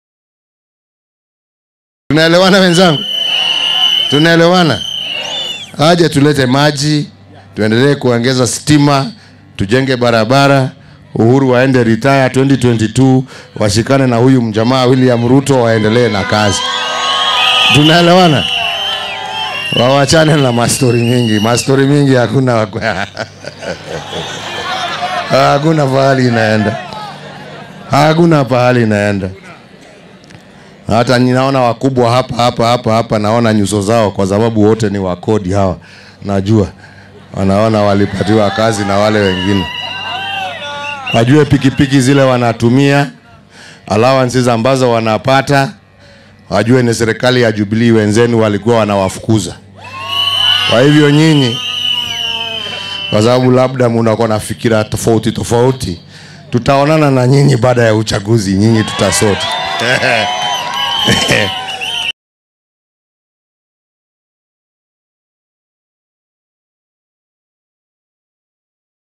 Stop helping Raila or we'll come after you, Uhuru warns Chiefs
President Uhuru Kenyatta yesterday warned chief in Makueni county that they will deal with them for supporting the opposition.